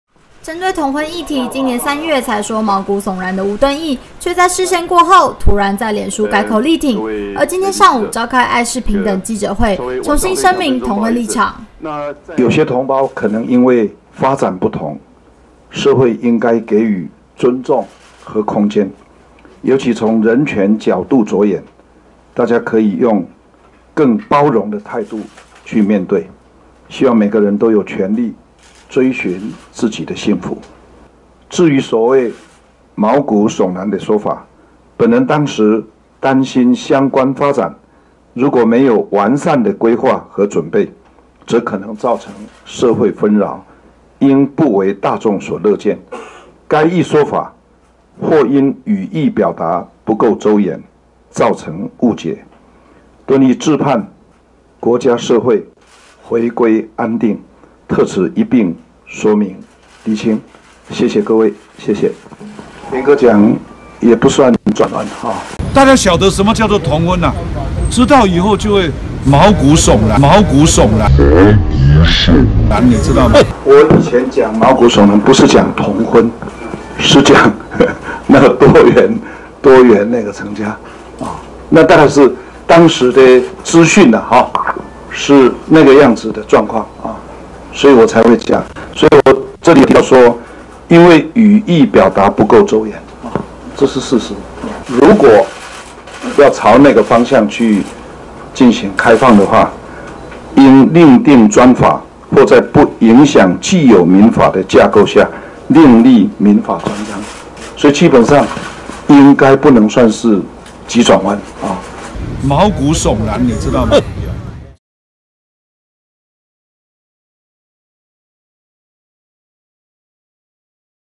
吳敦義：稱「毛骨悚然」是擔心規劃配套不足造成社會紛擾
吳敦義「毛骨悚然」聲明稿 記者會前突改版！

由於在同婚議題上被外界解讀為政策轉彎的國民黨主席當選人吳敦義，今天上午召開「愛是平等」記者會，親上火線說明自己的立場，對於外界質疑他過去談同婚議題時曾以「毛骨悚然」形容，他今日特別解釋自己的前後談話，「嚴格講也不算轉彎」，因為過去他兩度接受媒體訪問時，有提及訂定專法或在不影響既有民法架構下訂立專章，而且他提到「毛骨悚然」時，不是針對同婚問題，而是「多元成家」。